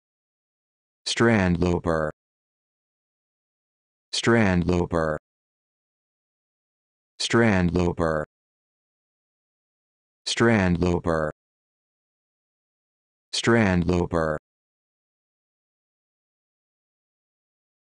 How to Pronounce Strandloper
This video shows you how to pronounce Strandloper